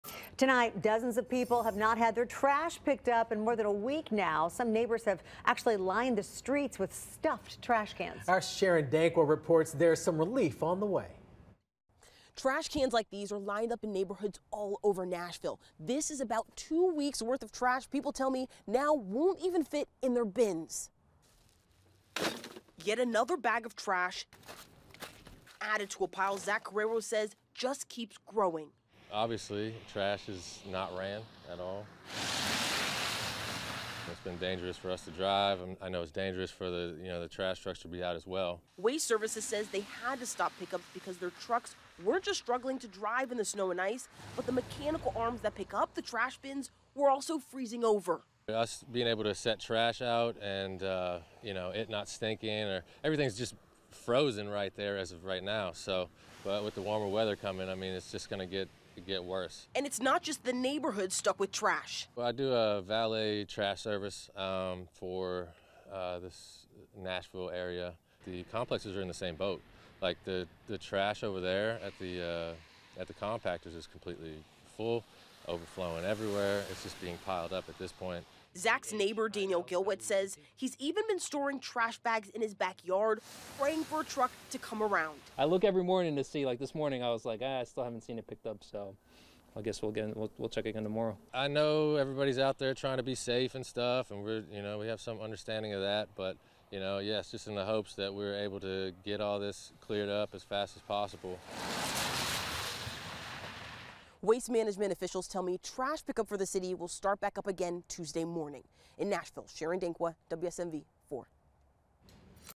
Trash & recycling collection resumes Tuesday
Dozens of people haven't had their trash picked up in over a week, but relief is on the way.